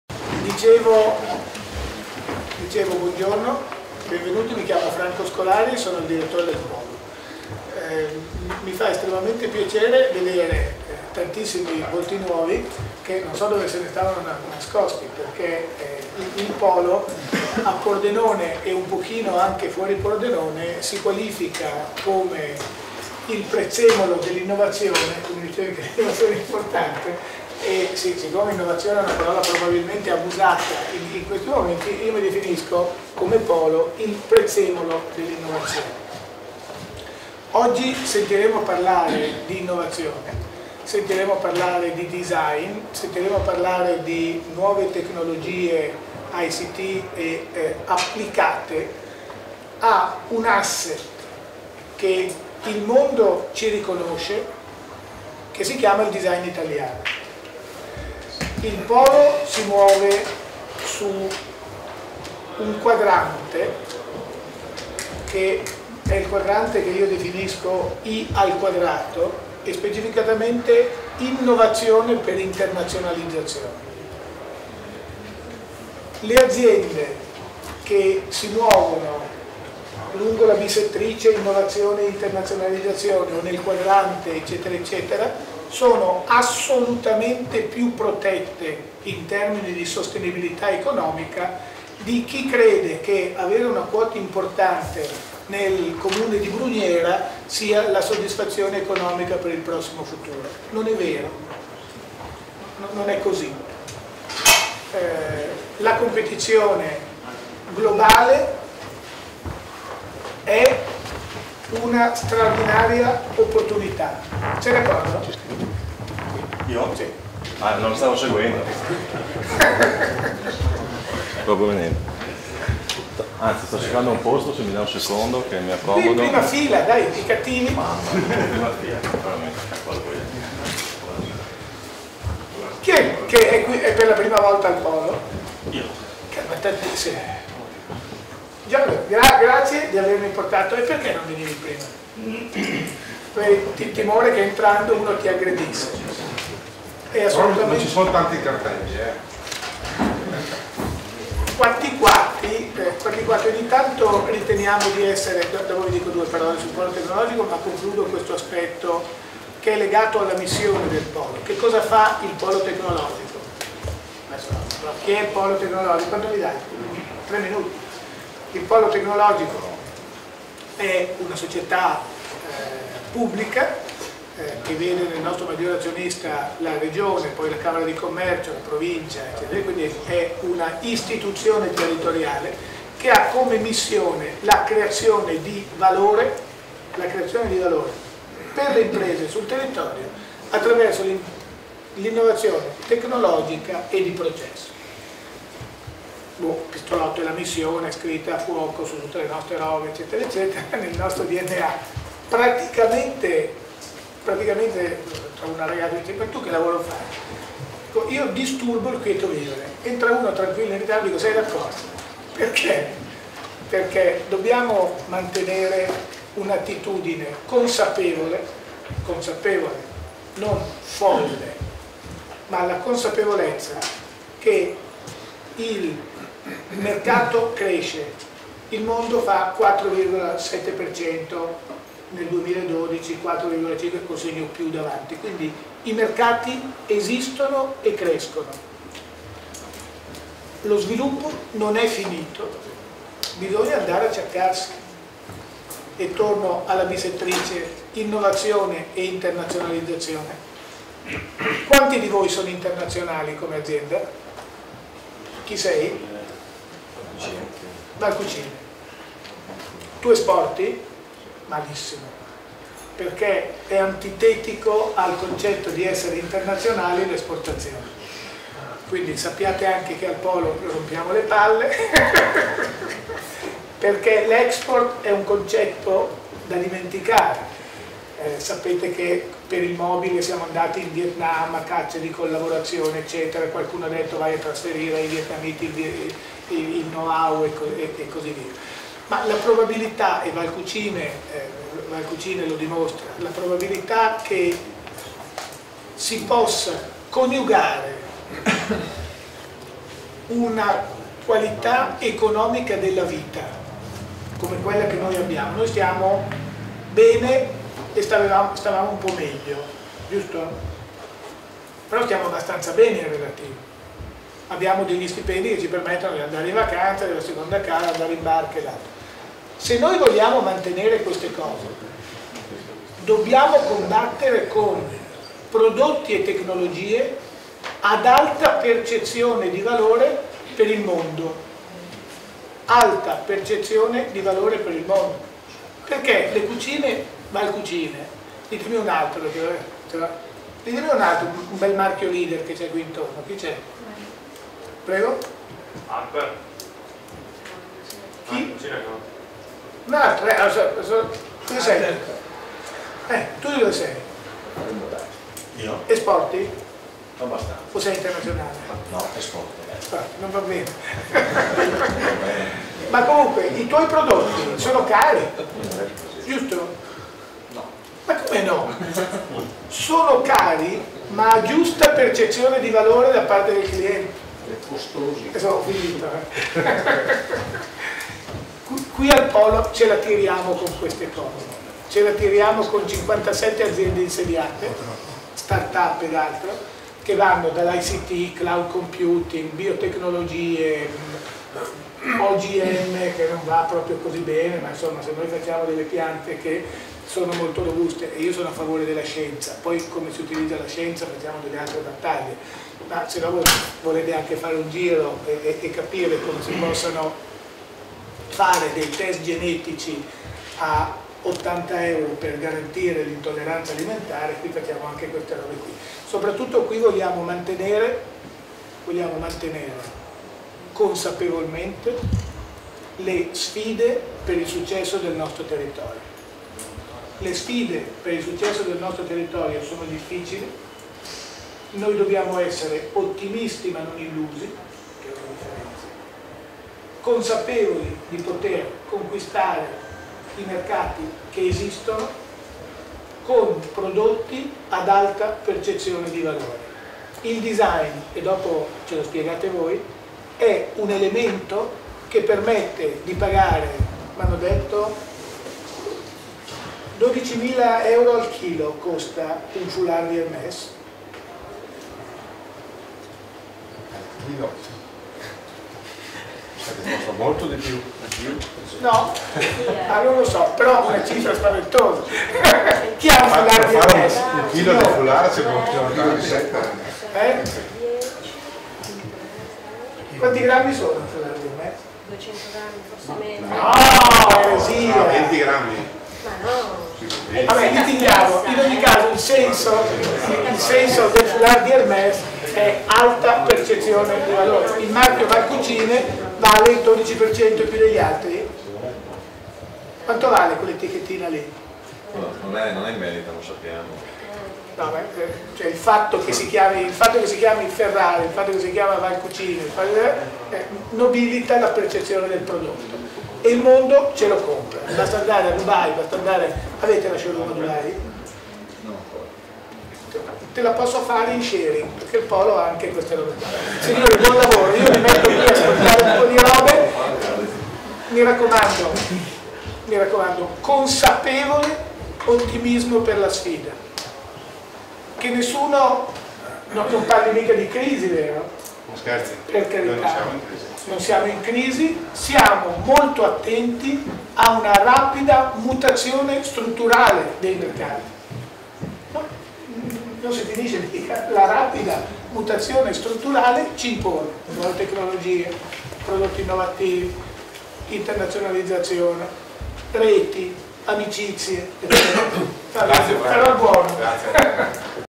L'evoluzione del mobile tra tradizione e innovazione - Franco scolari Polo Tecnologico PN
L'evoluzione del mobile tra tradizione ed innovazione
Si è tenuto il 04 Luglio 2013  presso il Polo Tecnologico di Pordenone un interessante incontro rivolto alle aziende del legno arredo e promosso da Prisma  Tech e Autodesk, dove sono state presentate delle novità e delle recenti applicazioni in ambito Autodesk rivolte proprio a questo settore.
Dopo il saluto del direttore del Polo Franco Scolari sono iniziati i lavori con le varie relazioni e testimonial.
In questa play list troverete tutti i video della giornata con tutti gli interventi.
y